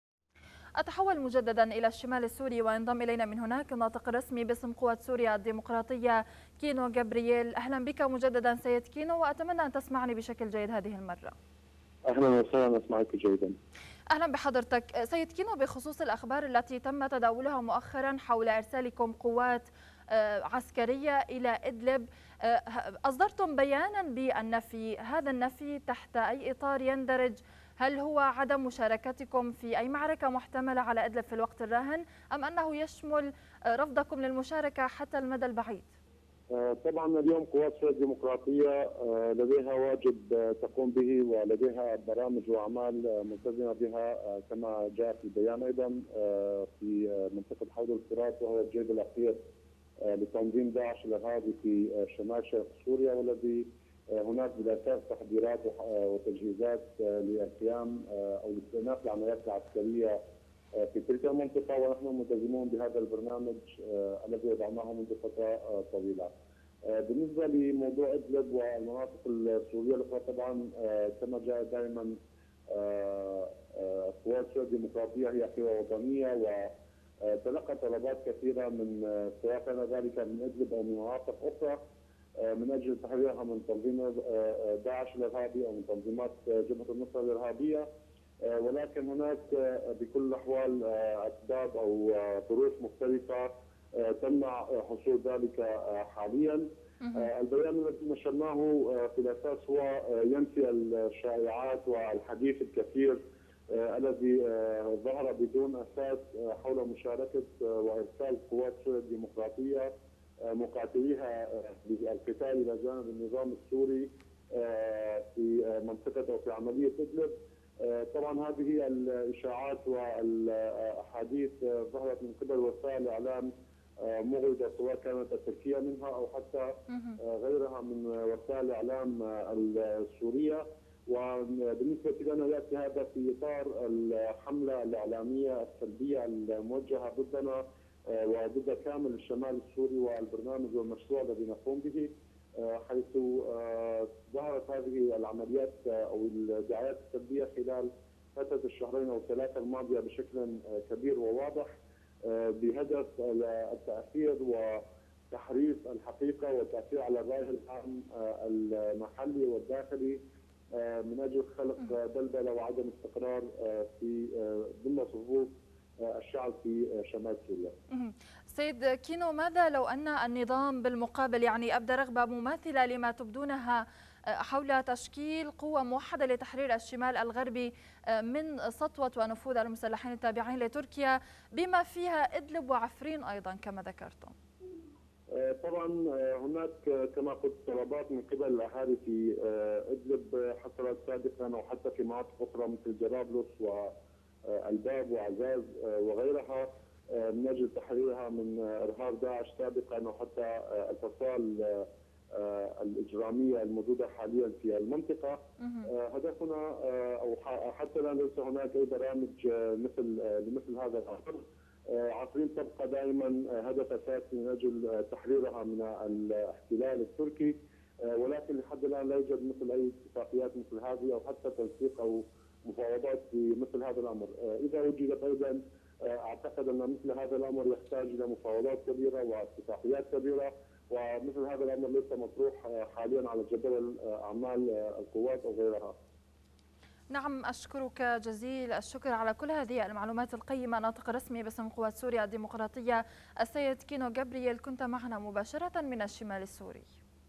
الأولوية لقوات سوريا الديمقراطية في الوقت الراهن دحر تنظيم داعش المتواجد شمال شرق سوريا
كينو كبرئيل ـ الناطق الرسمي باسم قوات سوريا الديمقراطية